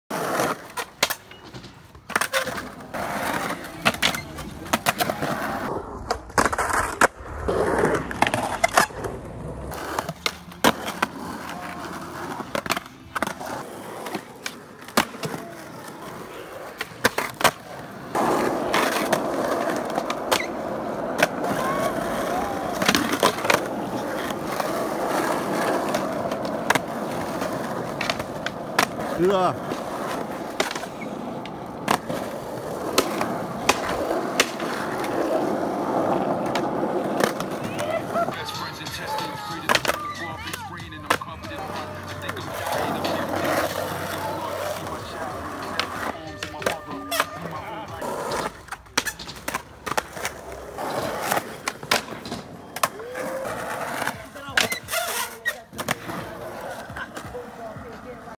Smørum Skatepark - Skateboarding
Troels S. Jørgensen og @claimerskaffe på det lokale spot i smørum. Skateboard klip, sommer 2020